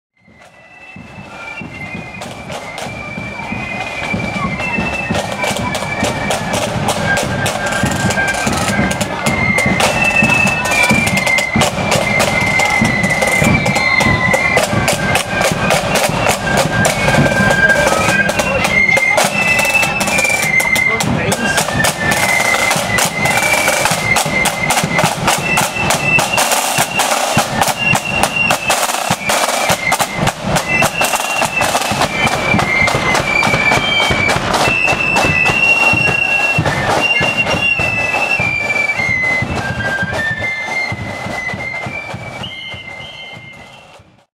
Pride of Ballybeen @ East Belfast Mini12th 2016
Pride of Ballybeen on the march at Ballymacarrett Somme Memorial Parade 1st July 2016